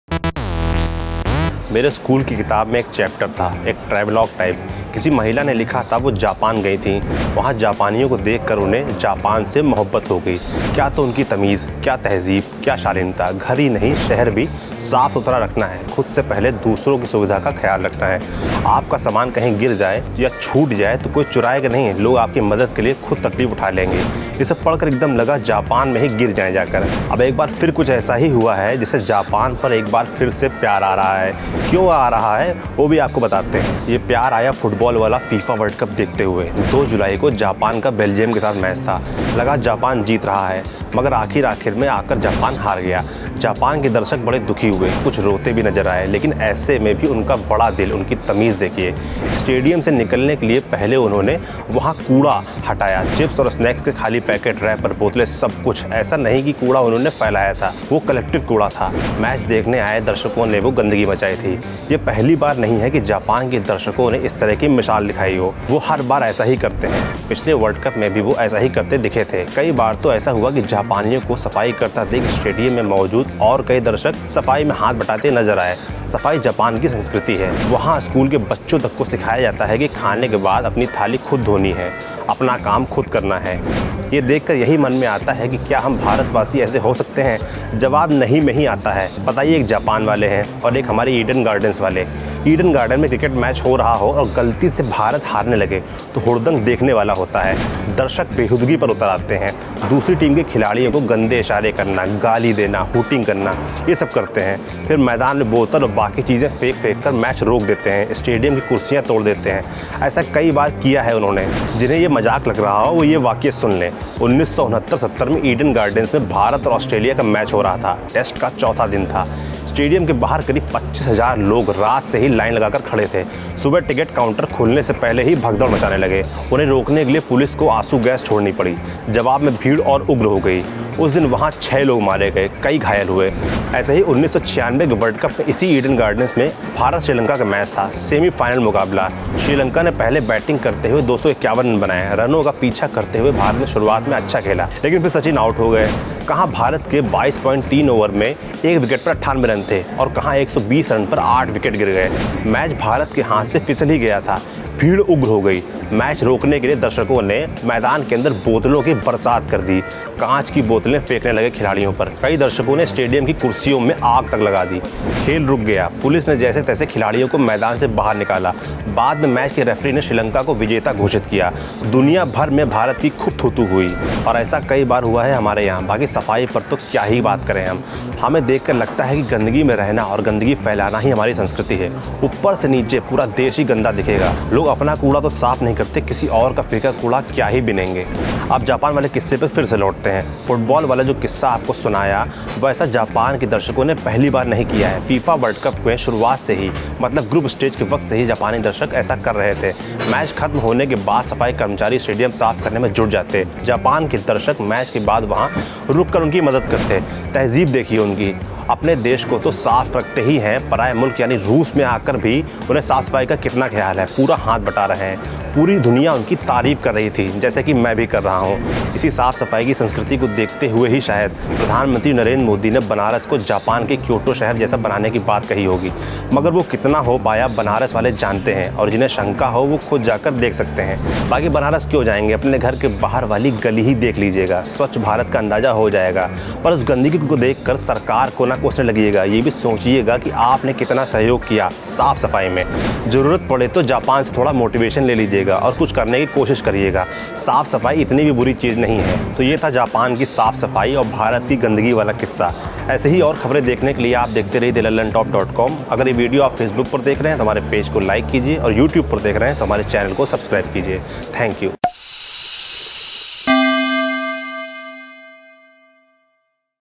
हिंदुस्तानी कभी वैसी तमीज नहीं दिखा सकते, जैसी Japan के लोग हारकर भी दिखा रहे हैं | Fifa World Cup
Japan loses to Belgium in FIFA World Cup 2018 yet Japanese fans clean stadium before leaving like they always do and Indians should learn from it.